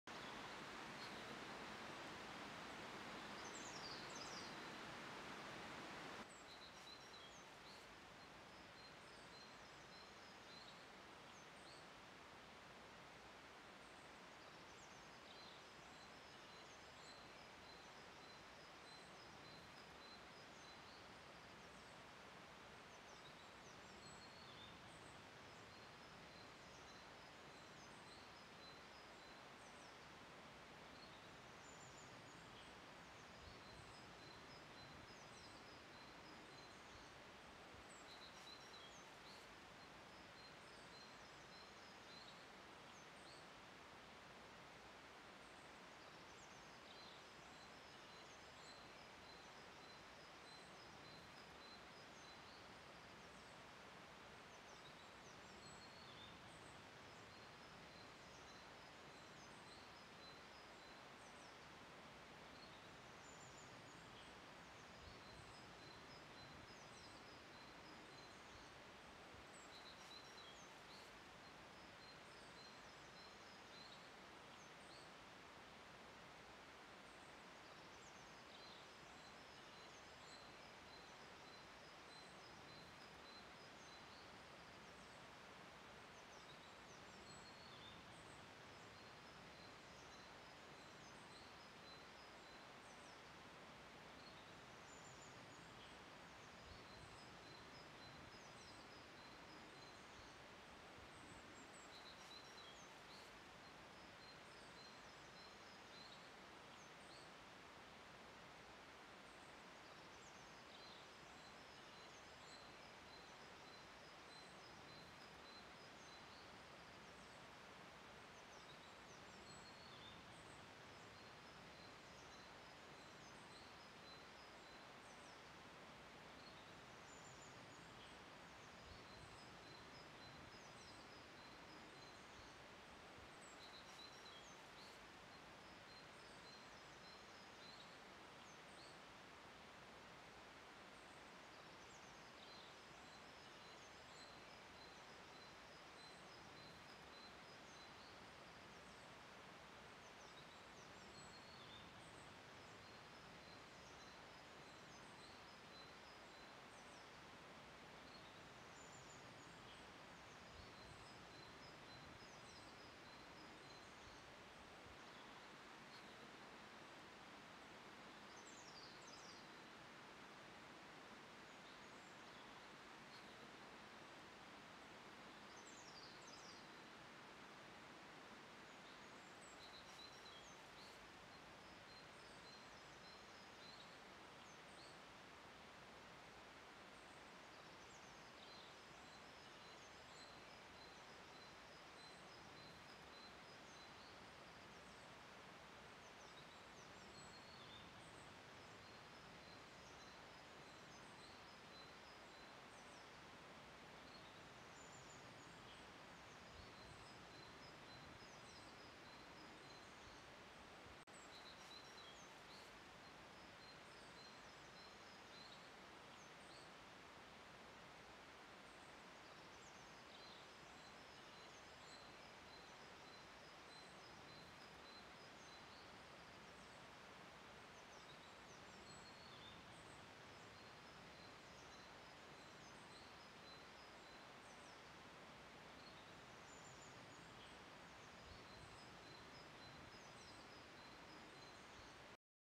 Nature
Man builds on greenbelt land such as parks and countryside that destroys the wildlife habitat. Everyday nature is becoming quieter.. For this project, I decided to visit two castles to see how nature eventually does claim back what man once destroyed.